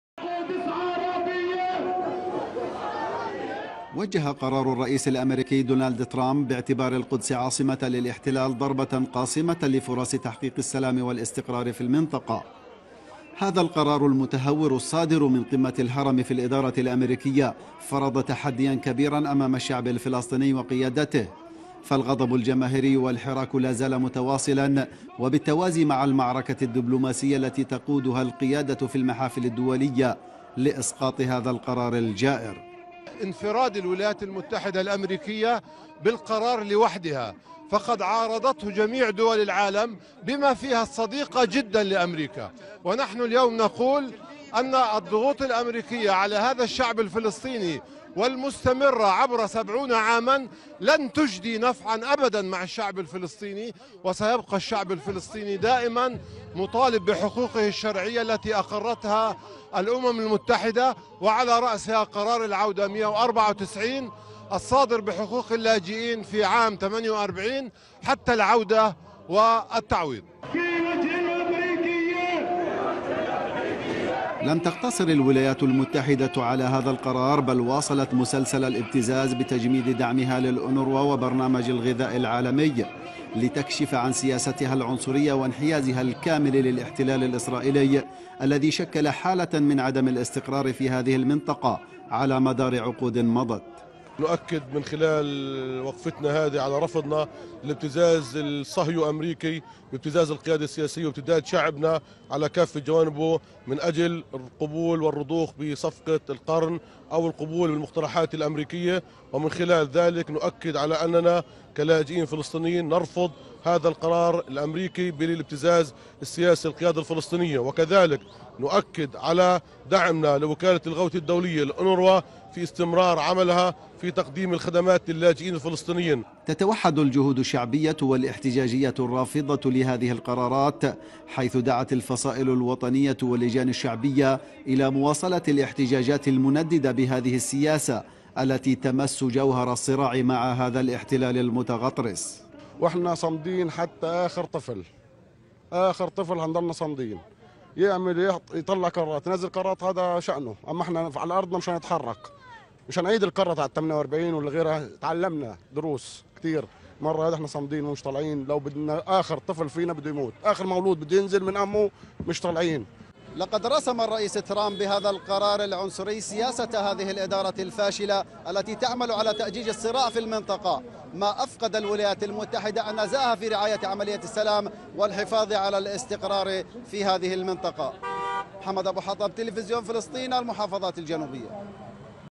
المحافظات الجنوبية | مسيرة جماهيرية في مخيم المغازي تنديداً بالسياسة الأمريكية تجاه القضية
المحافظات الجنوبية | مسيرة جماهيرية في مخيم المغازي تنديداً بالسياسة الأمريكية تجاه القضية الفلسطينية
تقرير: محمد أبو حطب